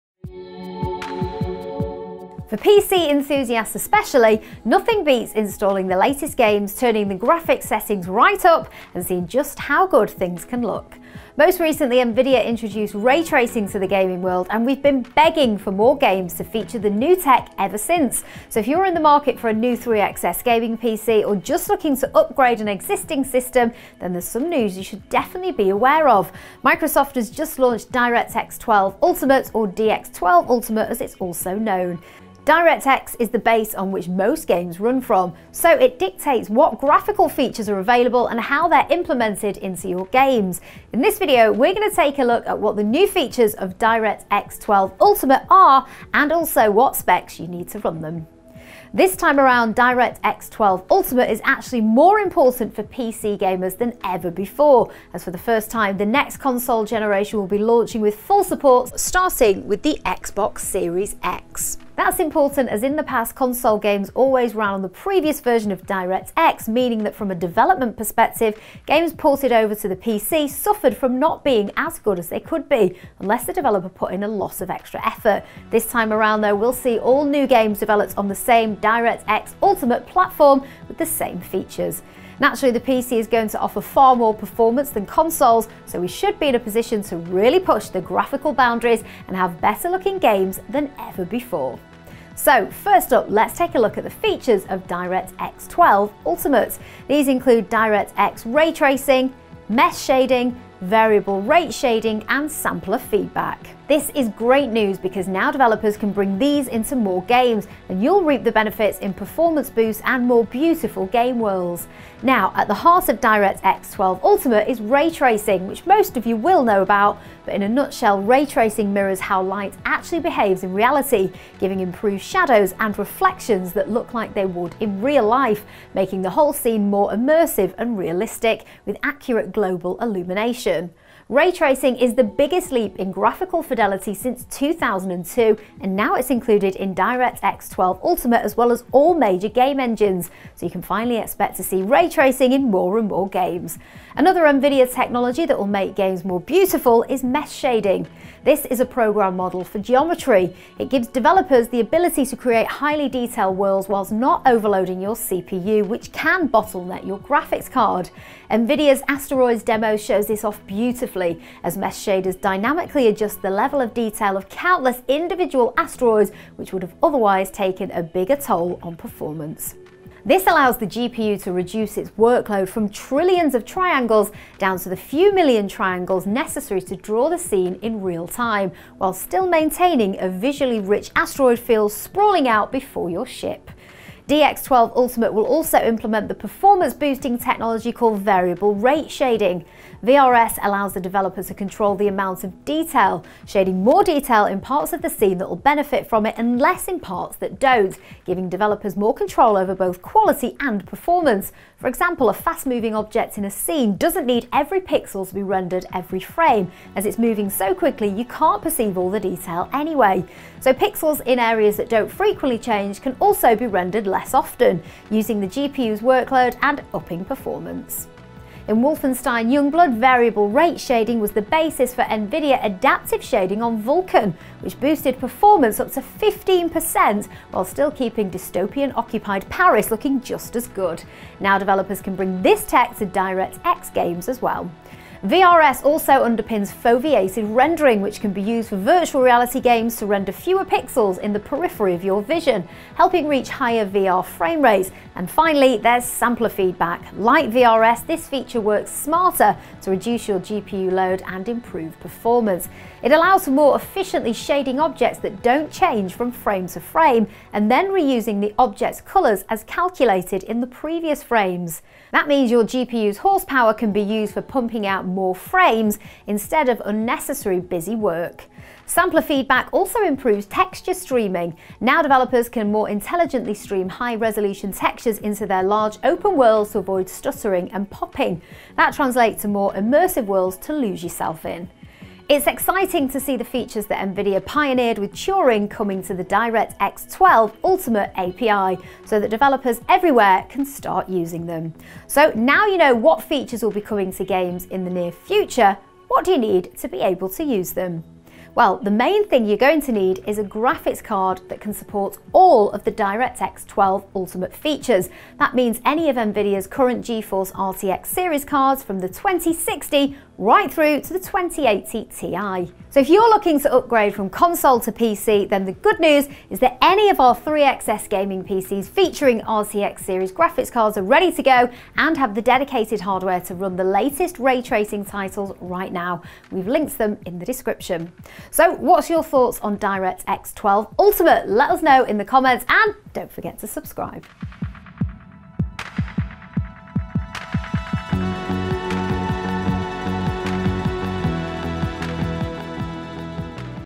DirectX 12 ULTIMATE Explained - Why its important to PC GAMERS! DX12 features Overview
Microsoft launched DirectX 12 Ultimate this year and it could be one of the most important releases this year. Not just because of the new features that allow PC graphics to take the next step in realism, but also because for the first time Both PC and console will be running off the same version of the platform. In previous generations consoles have always lagged behind PC's by as much as 2 generations, meaning that games designed for consoles first often didnt look as good as they potentially could when they came to the PC. Now that has all changed and features like Ray Tracing, Mesh shading and variable rate shading are all baked in ready for either platform.

In this video we take a look at what each of these features means for the future of games and how it will affect PC gamers going forward.